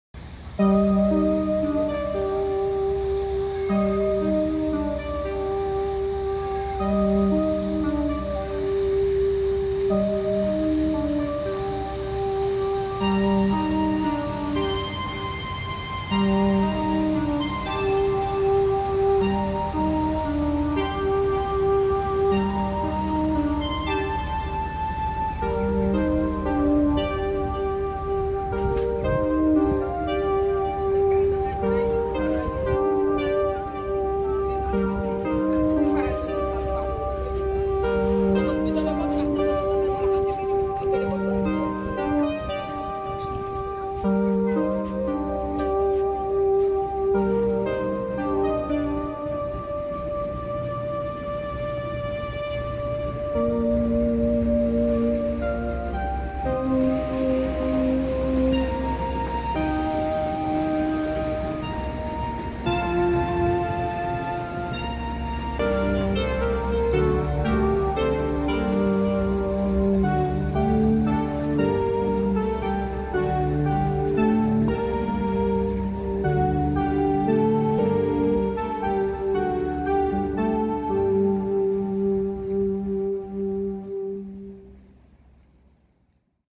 ship SHANNA 1960, Transport in Different Cities of the World. Dutch Bridge. River Travel Netherlands
1AnalogueCabin   Noir Et Blanc Vie a23 3a  a23 5a=
Every Saturday there is a New Video about Transport in Different Cities of the World.